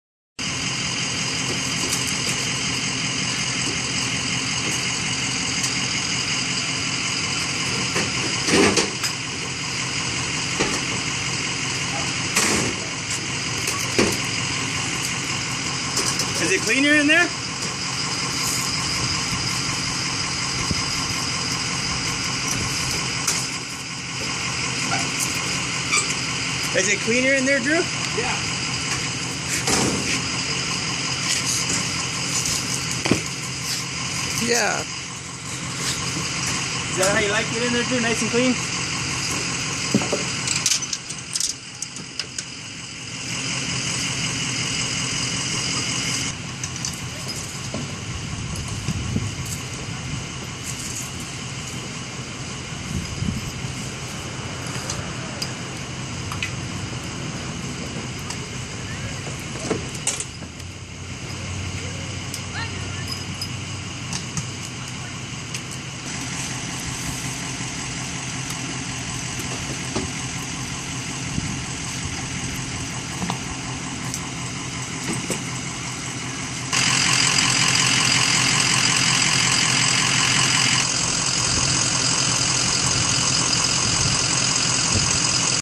Plumbing Fresno, Ca
Eddie and Andrew working to restore a main sewer line with a hydro jetter.